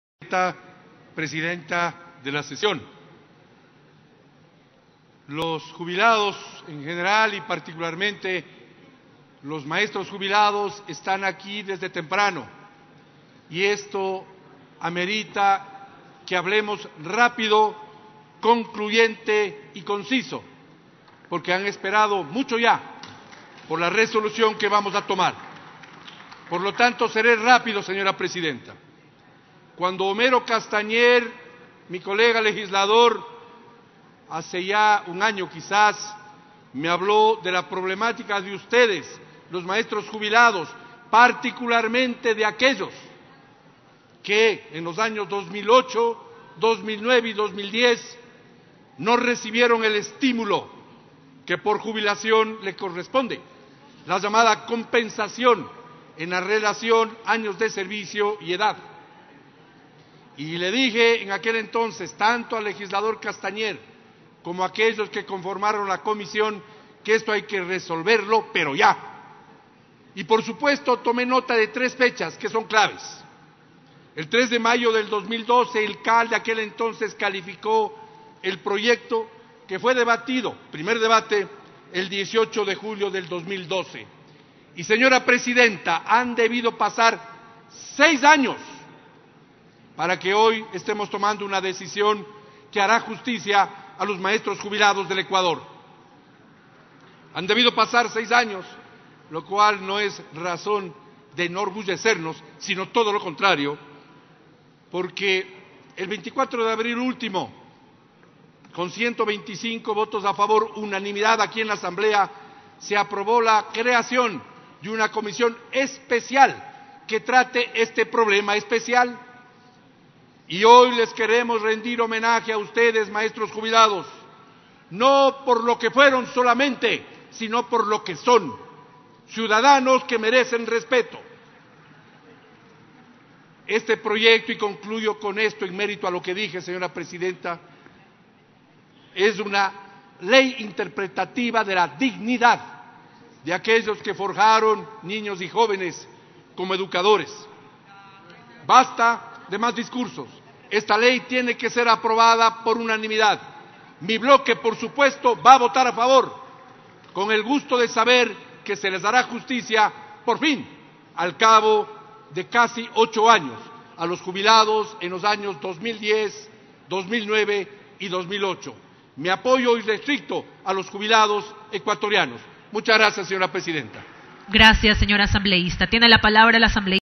Patricio Donoso - Sesión 552
13 de noviembre de 2018
Conocer y resolver sobre el Informe para Segundo Debate del Proyecto de Ley Orgánica Interpretativa a la Disposición General Novena de la Ley Orgánica de Educación Intercultural.